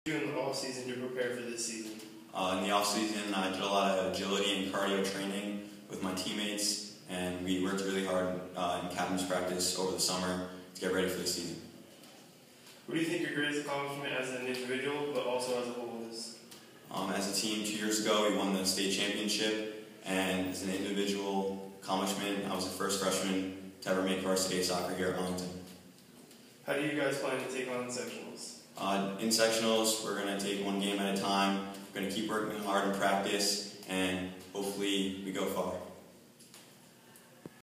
Athlete of the Month - Vinny Colantuono
We interviewed the male Athlete of the Month for September, soccer player Vinny Colantuono.